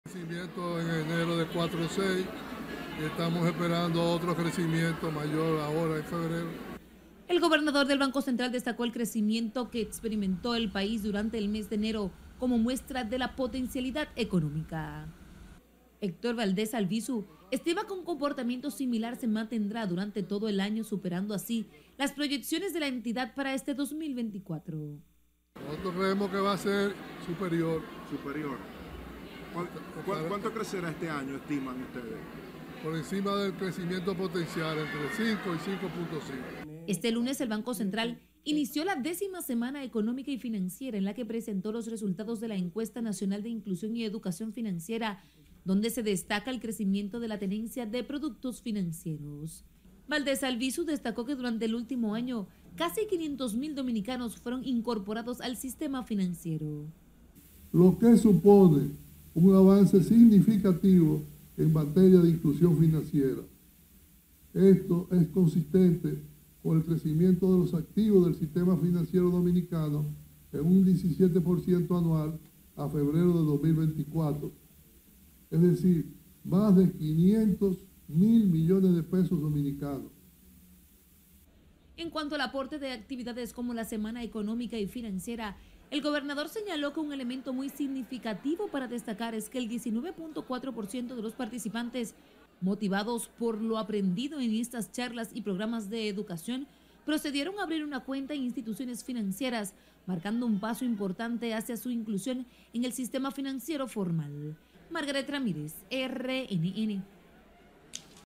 BCRD proyecta un crecimiento económico de más de un 5% en el 2024
El Gobernador del Banco Central, Héctor Valdez Albizu, aseguró este lunes que se mantiene una proyección de crecimiento económico por encima del potencial estimado de entre 5 y 5.5 por ciento para este 2024.